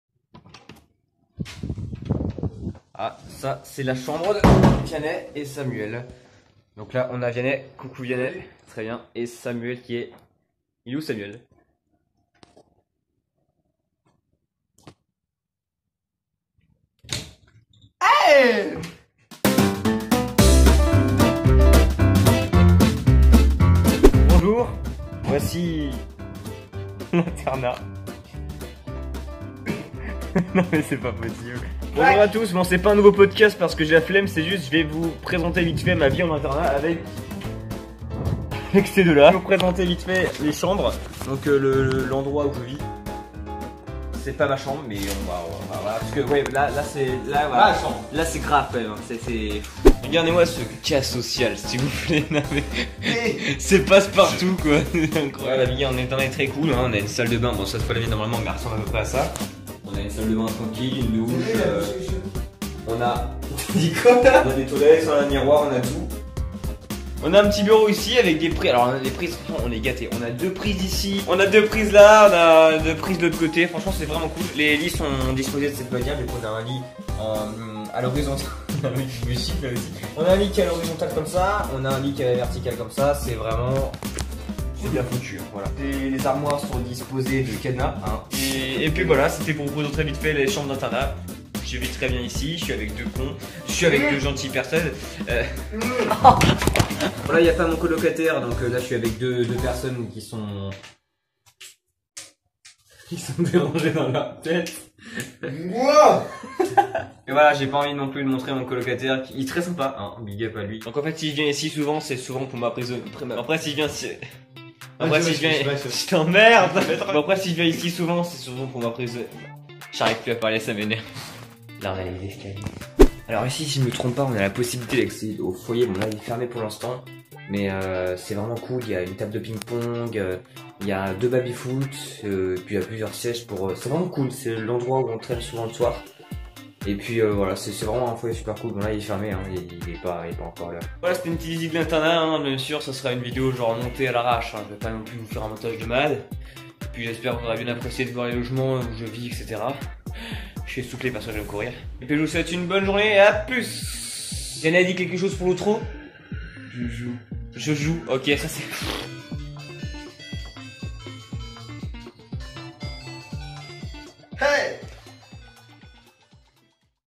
Visite de l'internat
Bonjour à tous, petite vidéo pour vous montrer que j'suis pas un mytho et que je suis bien en internat x)
On s'est bien tapé des barres, j'espère que vous serez accueillant avec Vianney et Samuel qui apparaîtront peut être dans la suite des vidéos quand j'aurais pas trop de travail à faire !
Bon visionnage